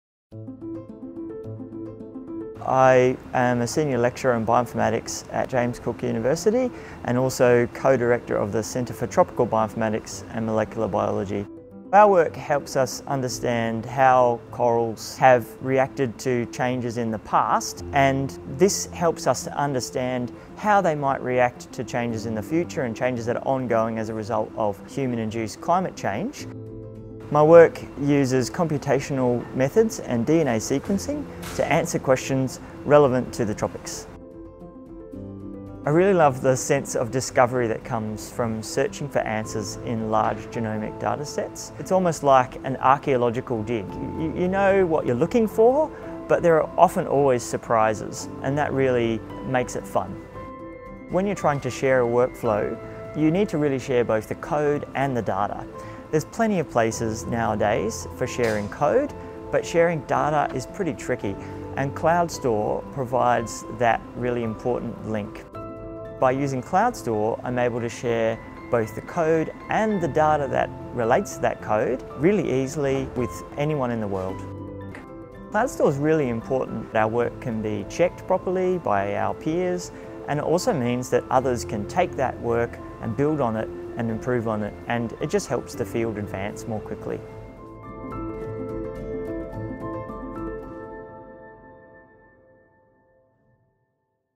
James Cook University - AARNet CloudStor User Testimonial
Please note: CloudStor was decommissioned in December 2023.

CloudStor was a collaboration workspace with integrated tools for sharing and analysing data. It was developed by AARNet to meet the unique needs of the research and academic community.

Find out more about AARNet and the services we provide to help researchers at aarnet.edu.au/research